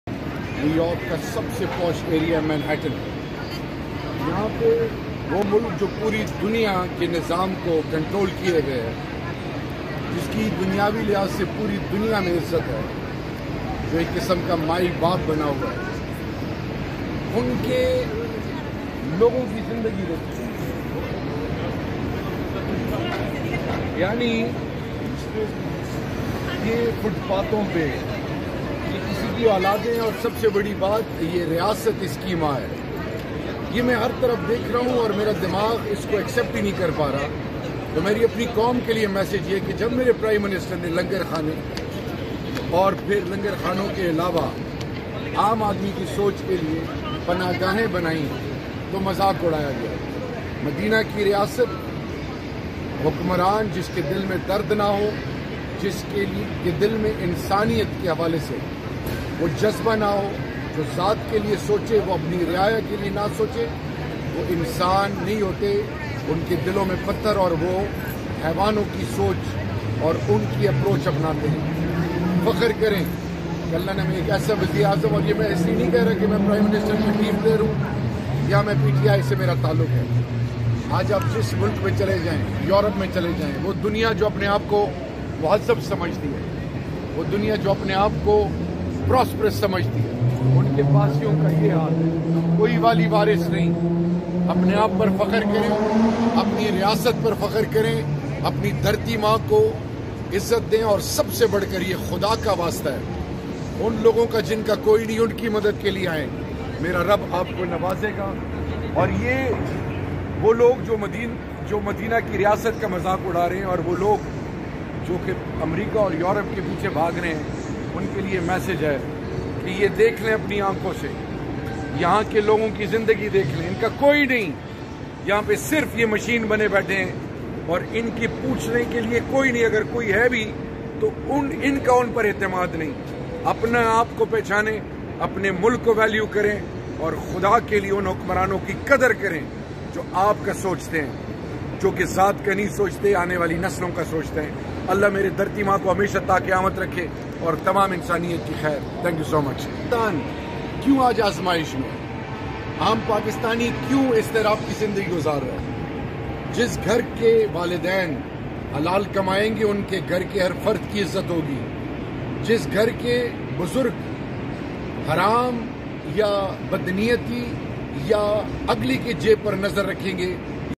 Sheheryar Afridi Federal Minister very sad after seeing homeless peoples on road New York Manhattan
Federal minister Shehryar Afridi sad to see homeless people on road @ Manhattan New York